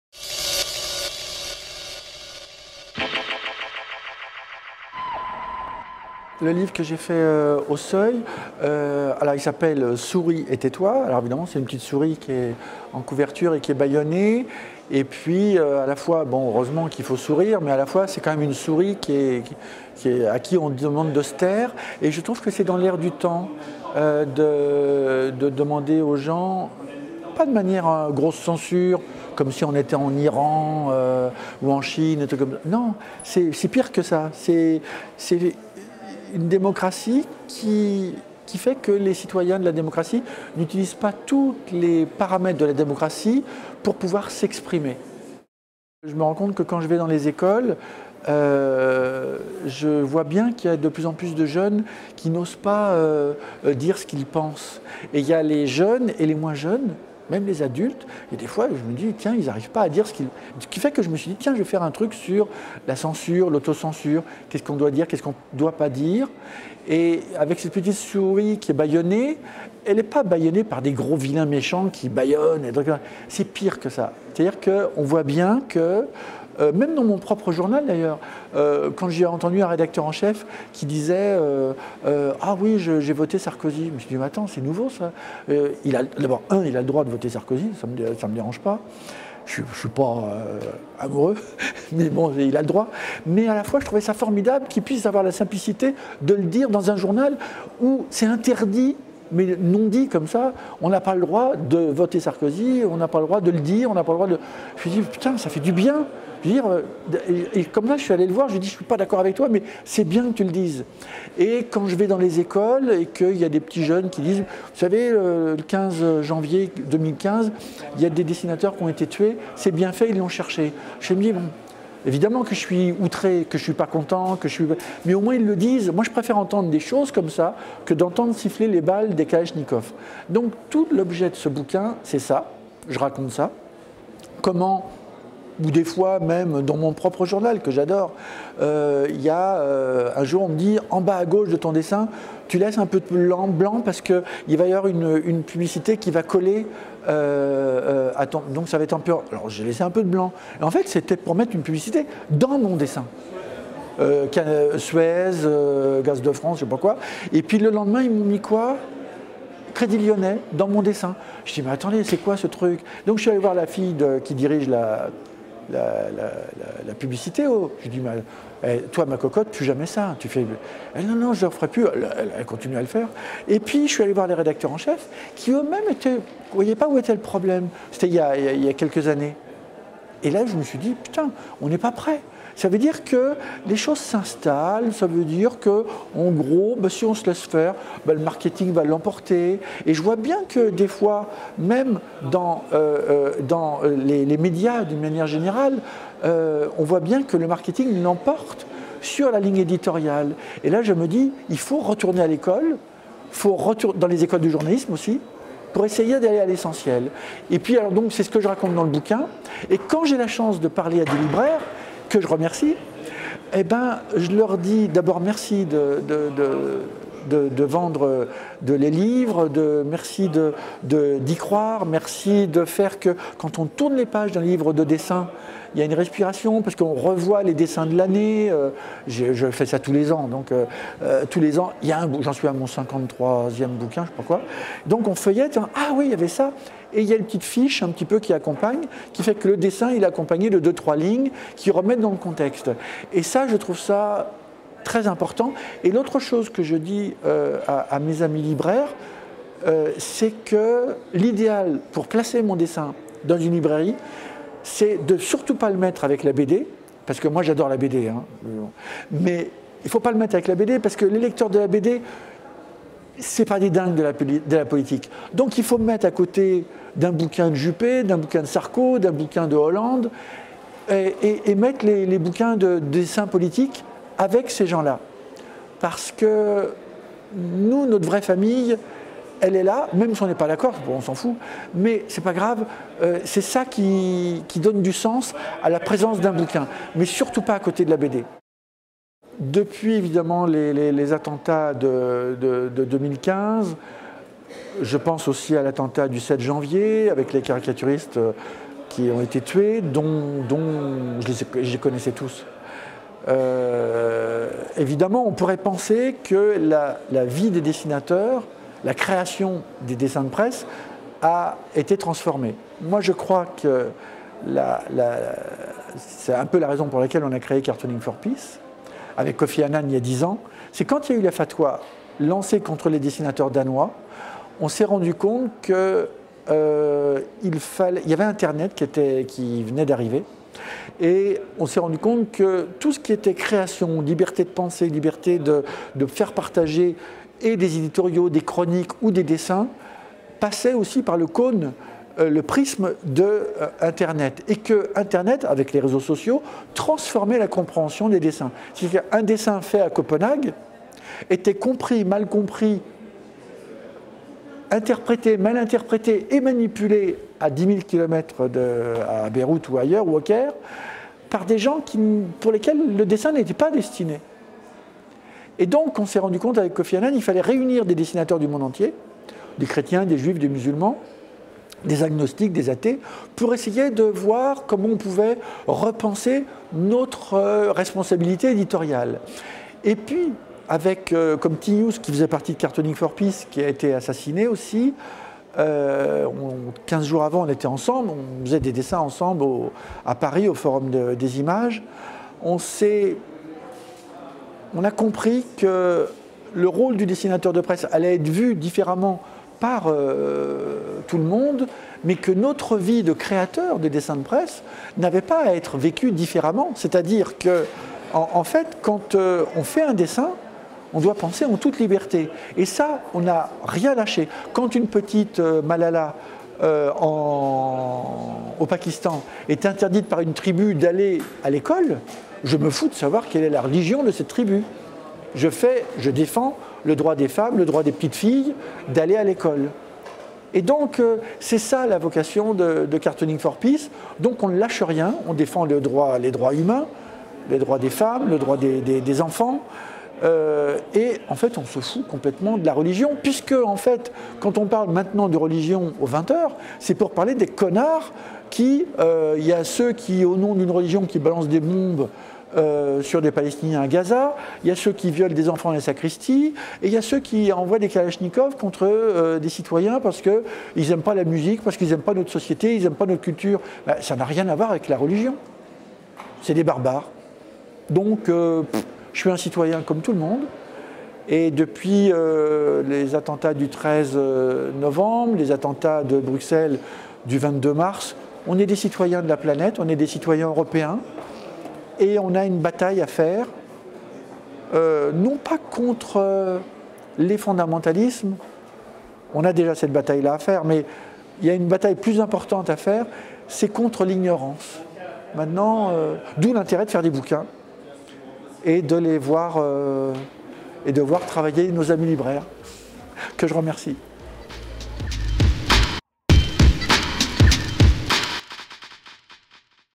Plantu - Souris et tais-toi !
Plantu vous présente son ouvrage "Souris et tais-toi !" aux éditions Seuil.